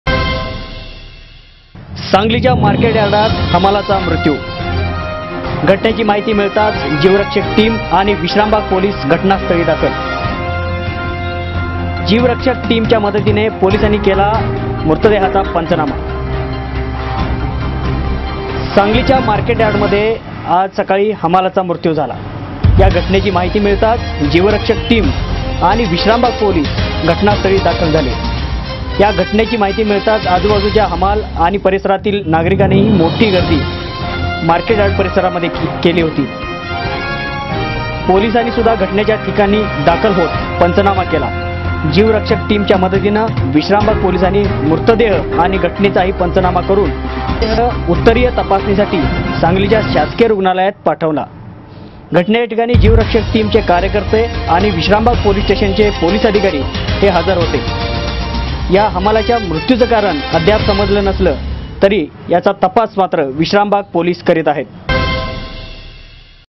YOUTH LEADER NEWS
सांगलीच्या मार्केट यार्डात हमालाचा मृत्यू